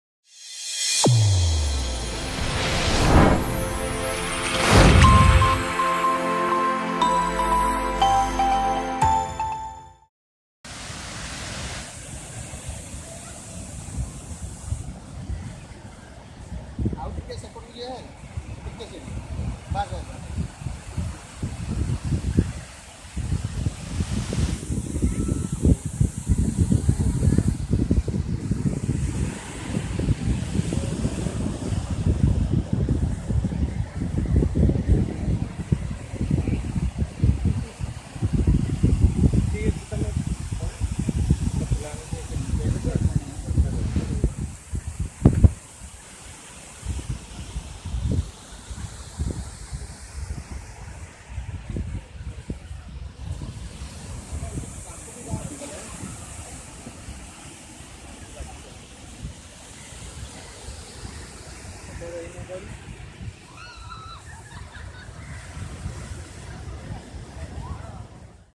Sand Art tribute to Sridevi on Puri beach by Sudarsan Pattnaik
Internationally acalimed sand artist Sudarsan Pattnaik pays tribute to Sridevi  on Puri beach through a sand art.